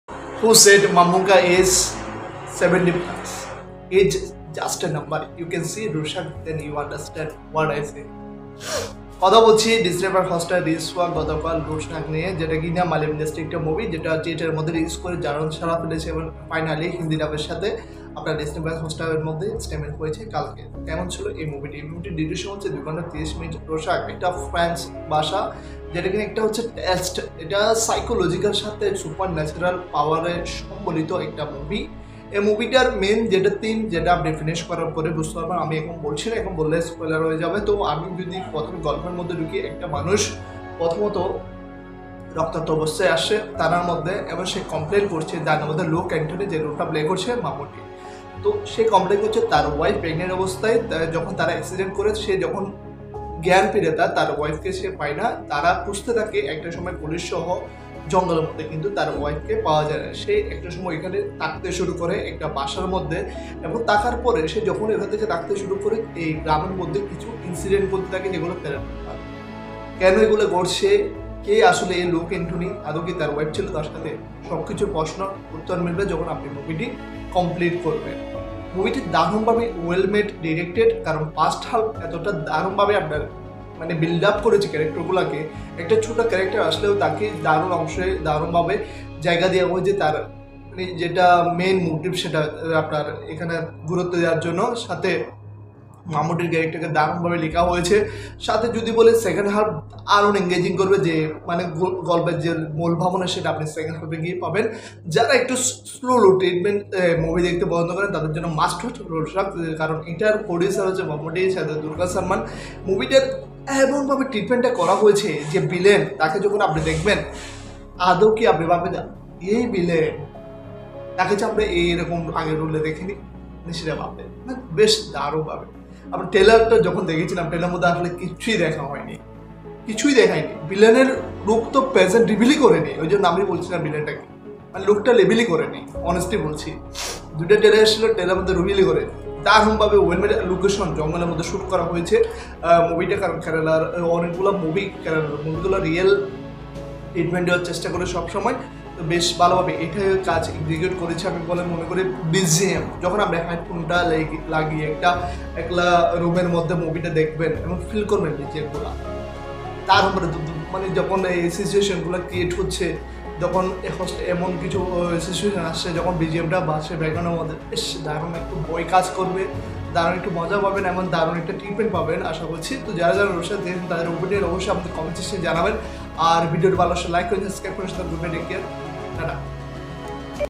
Rorschach Full Malayalam Movie Review In Bangla By Saiful Asif, Mammootty & Asif Ali,TheConfused Man
PRESENTED BY SAIFUL ASIF

The Movie is about Luke Anthony who has a mysterious past and is out on a mission to seek revenge from someone who has destroyed him deeply

IMSTA ID - saiful_asif